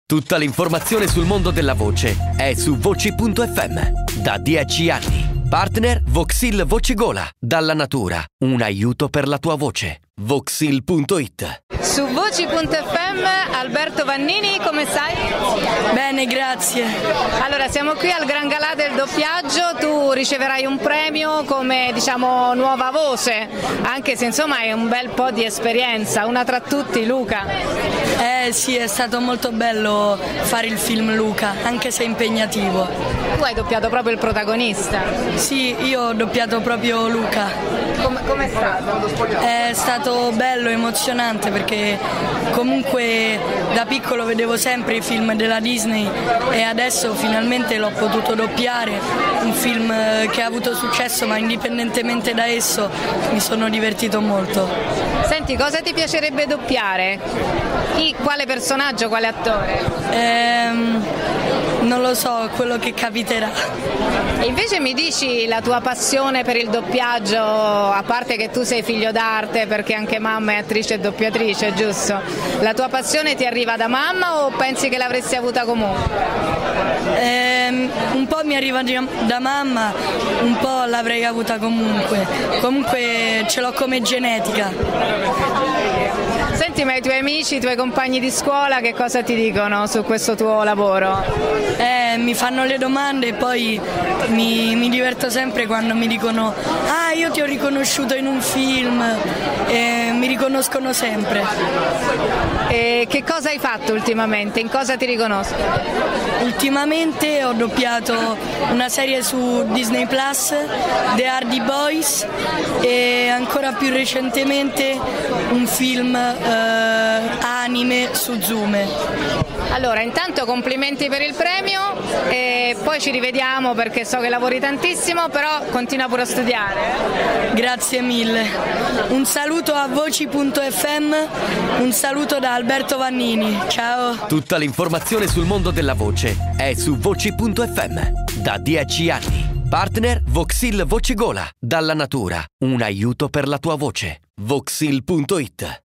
Gran Galà del Doppiaggio 2023: Alberto Vannini su VOCI.fm
Gran Galà del Doppiaggio 2023: intervista al doppiatore Alberto Vannini.
A cura di Patrizia Simonetti per il magazine VOCI.fm